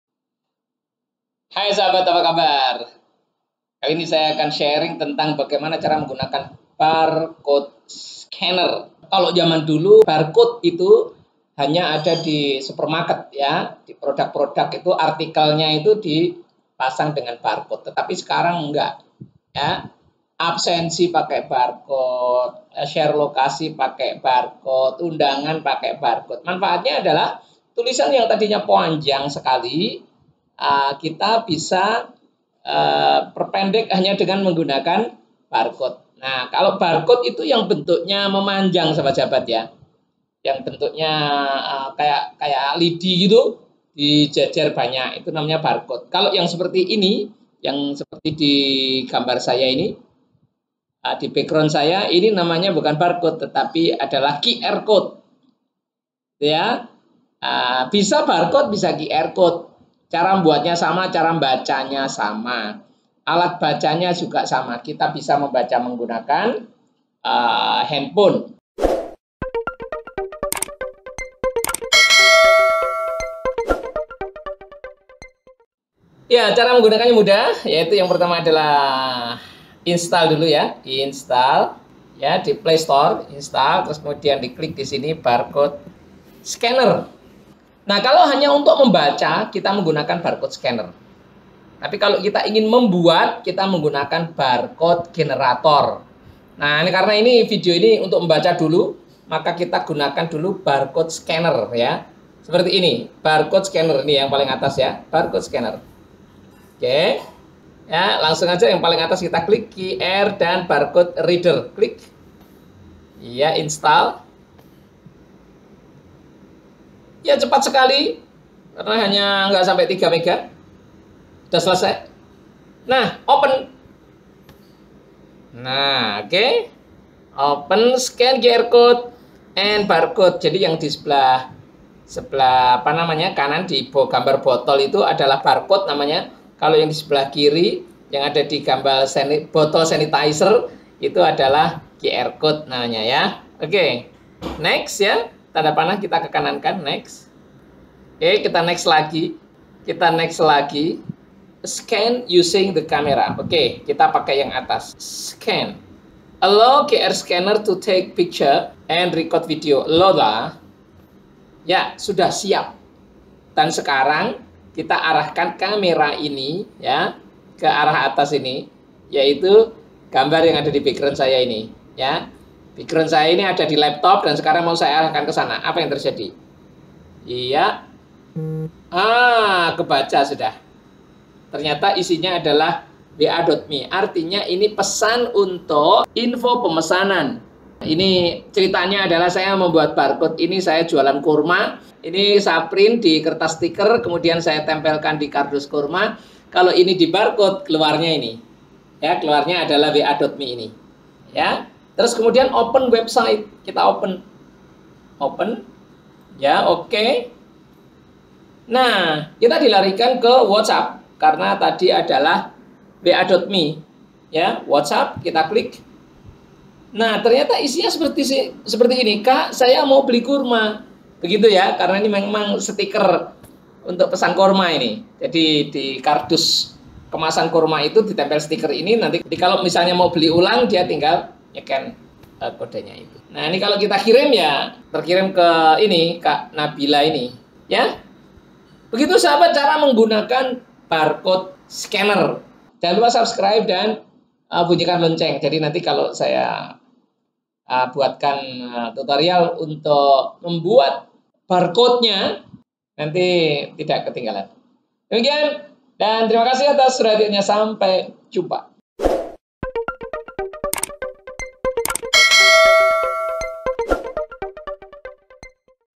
CARA PAKAI BARCODE SCANNER DI HP ANDROID
CARA PAKAI BARCODE SCANNER DI HP ANDROID. Sekarang saya jelaskan dahulu bagaimana install dan menggunakan barcode scanner untuk membaca barcode/ QR Code di android. Video berikutnya bagaimana cara membuat barcode itu sendiri.